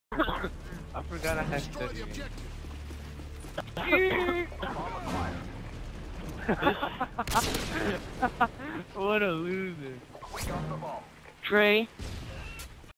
My First Spawn Shot
My first spawn shot but it shows I blew something up to kill him.. but Idk what happened.. :D